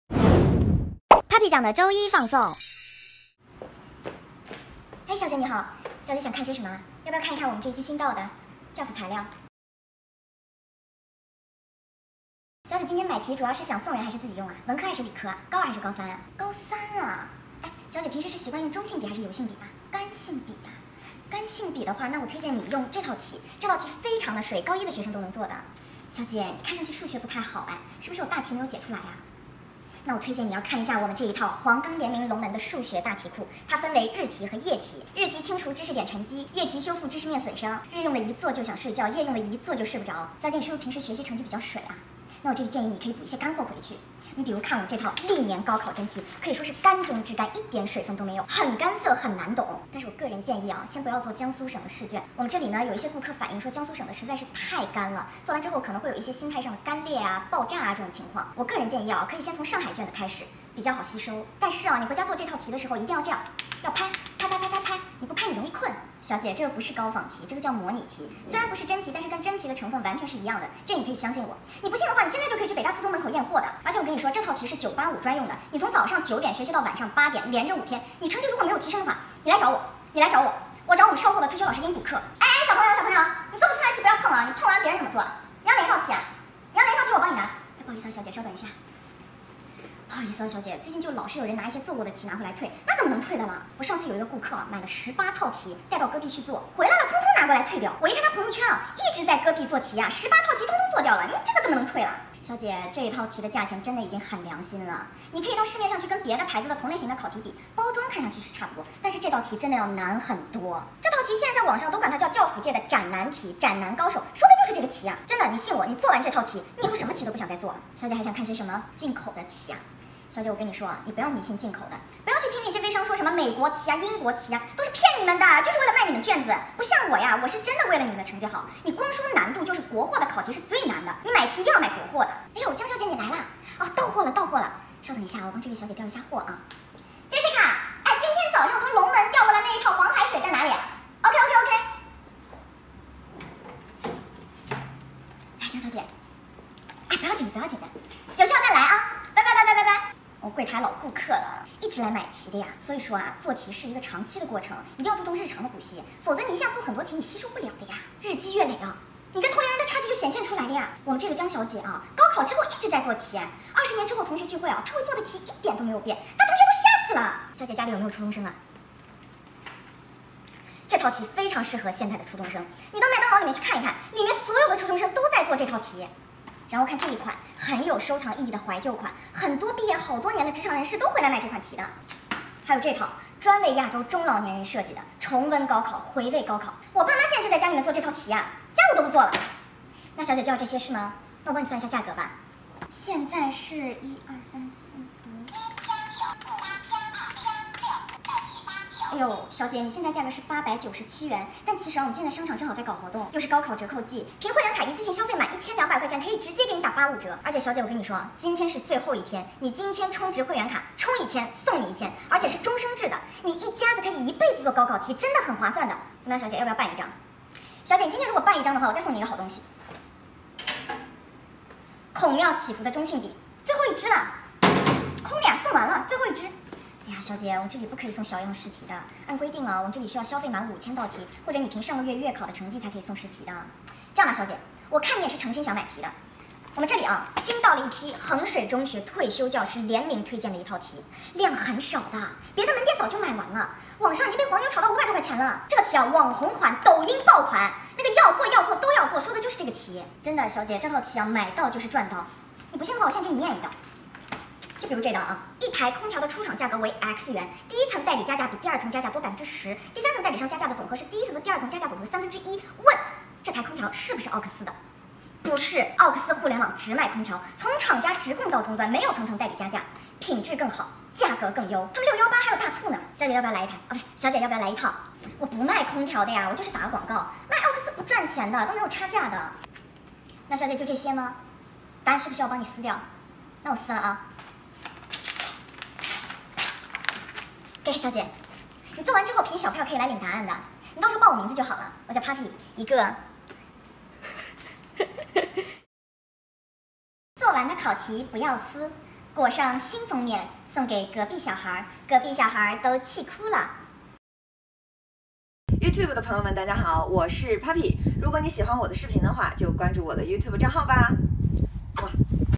papi酱 - 如果化妆品专柜售货员，转行去卖教辅材料的话…【papi酱的周一放送】
papi酱的周一放送——这位顾客您好，今天想来买点什么？要不要看下本季最新到货的，教辅材料？（祝愿即将高考的的学子们考出自己理想的成绩~（已经通过高考的也可以继续做题，重温高考，回味高考哦~